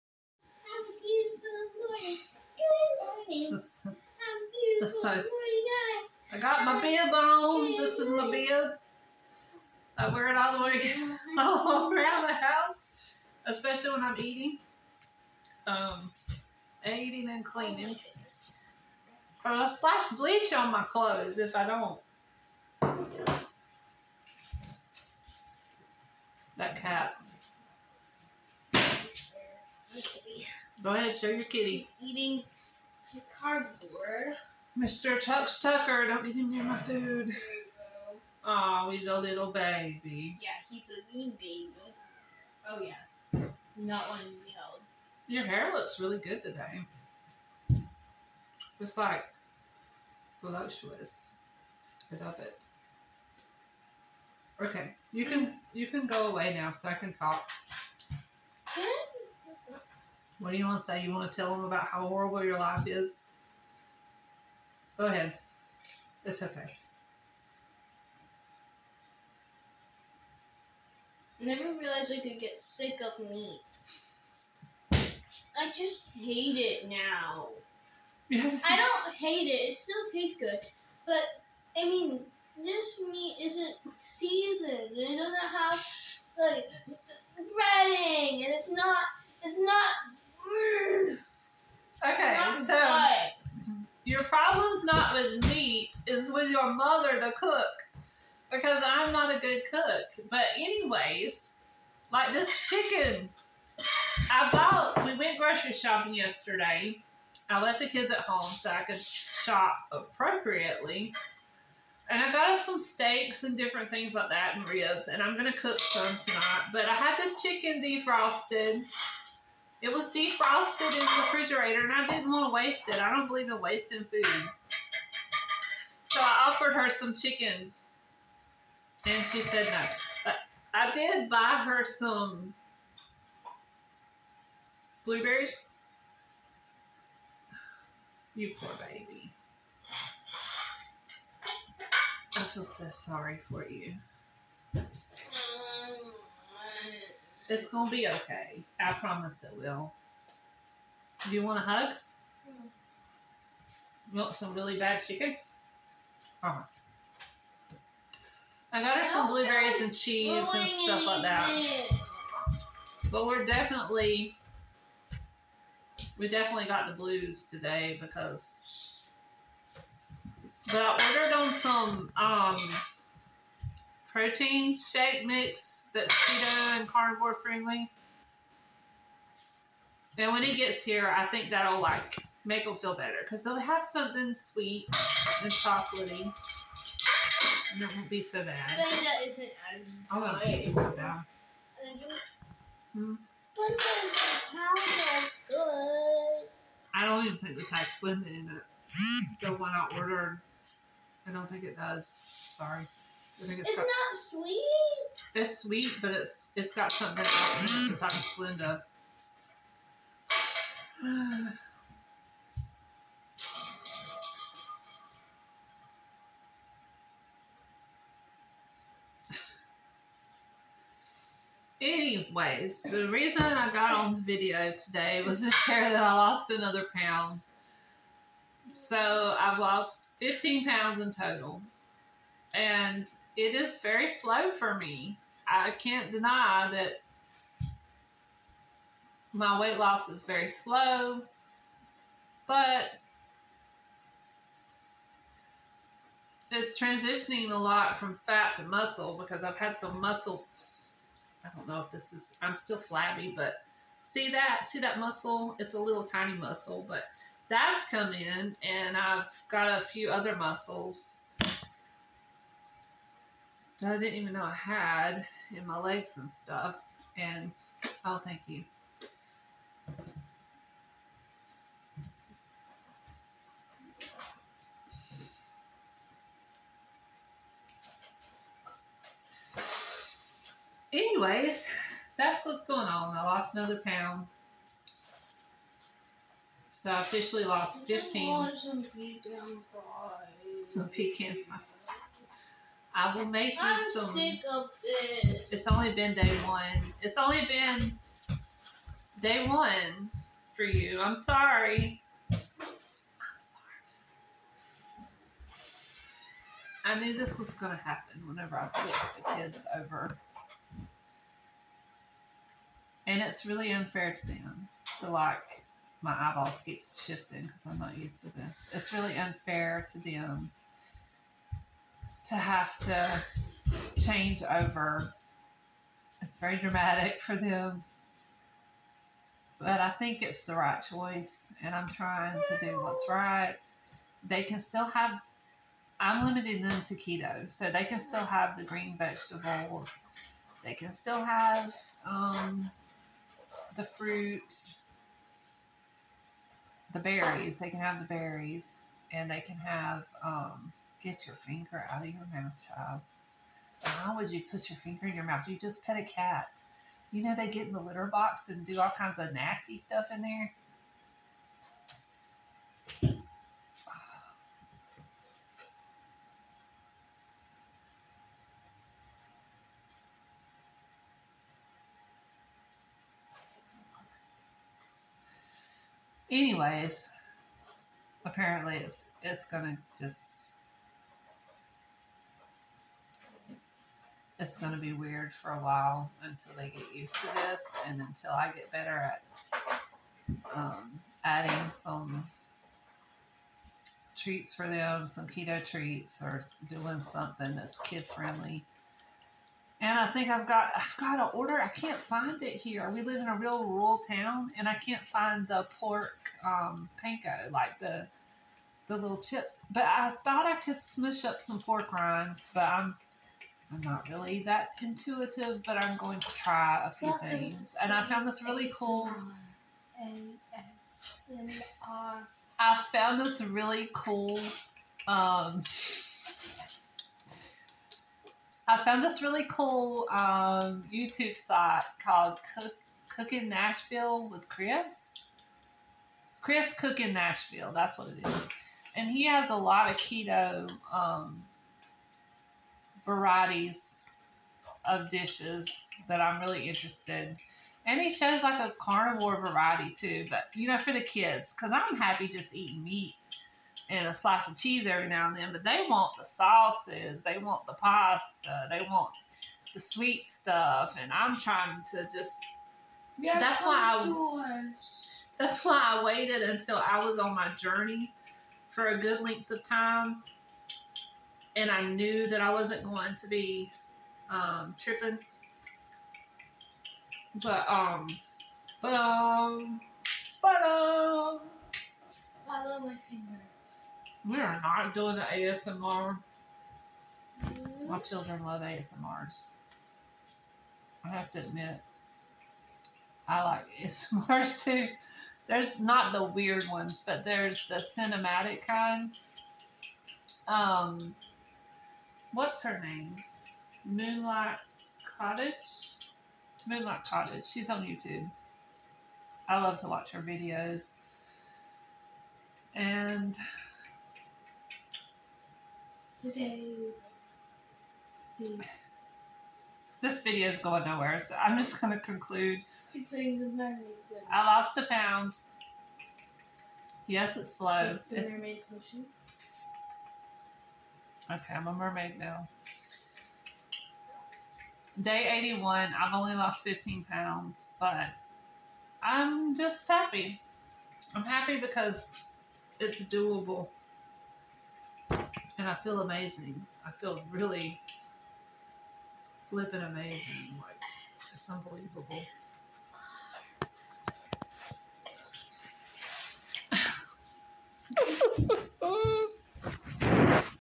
Day 81 for me. Day 1 for the kiddos
My youngest tries to asmr my face while I am talking. 😂  Today is day one keto/ carnivore for them.It ain't all fun and games.